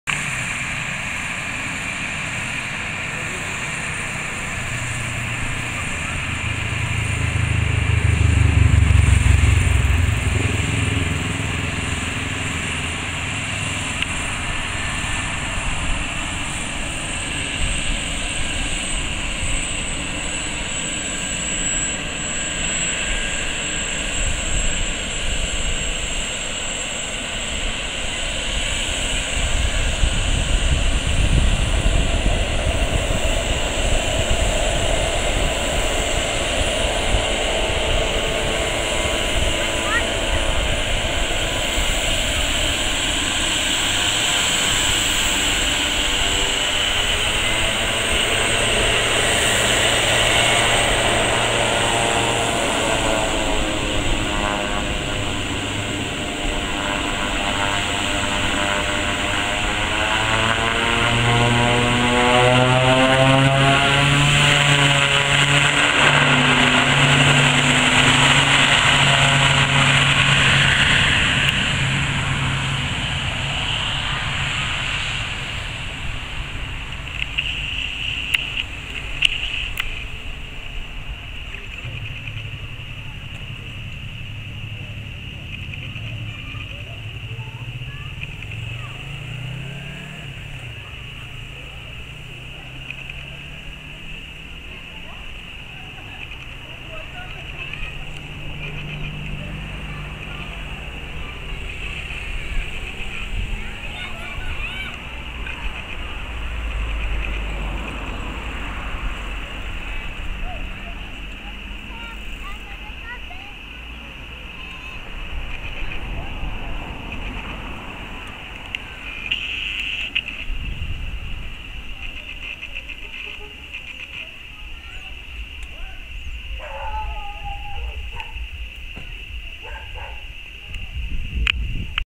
Decolagem Pilatus PC 12NG PR - CBJ de SDAG
Pilatus PC 12 47/E da Icon Táxi Aéreo com destino a SBSP